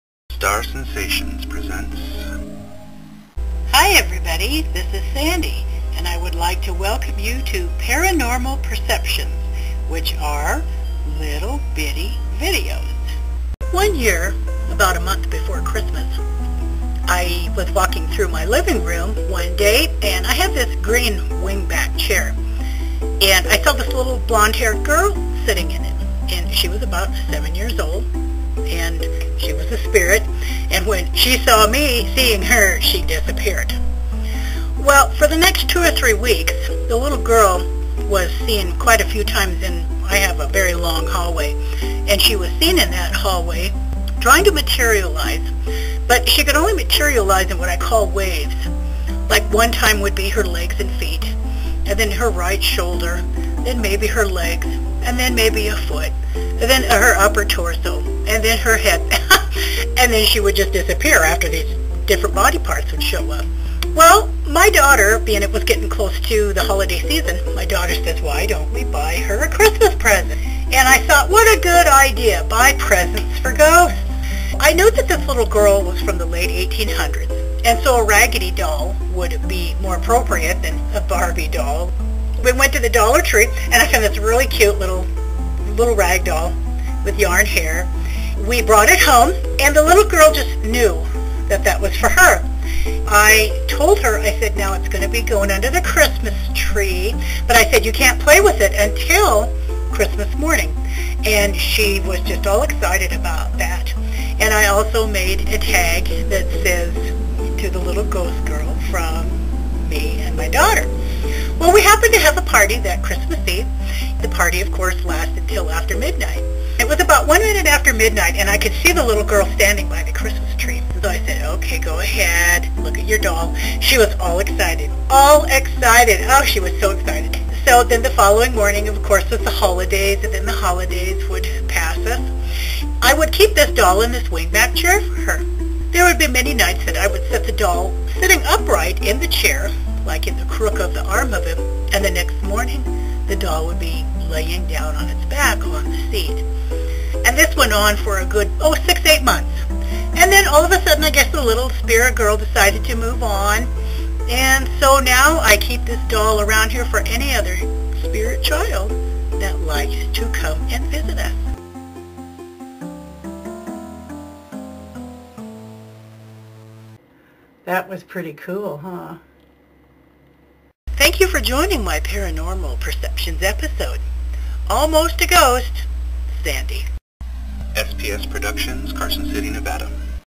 Little Ghost Girl with Doll
This video is about a young girl spirit in my home, who received a Christmas Gift!!